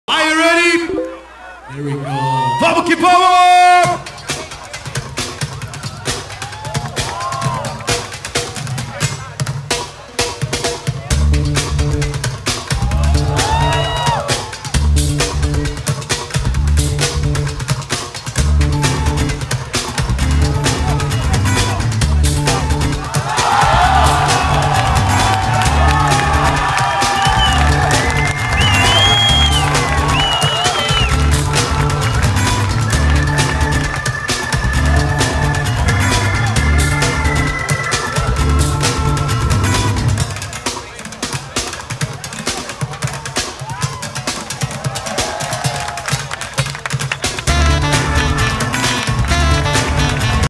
Rocko Rovira / AOM Brazil 2011
From México: Rocko's Run in the Art Of Motion in Brazil Frapsed from the Livestreaming online.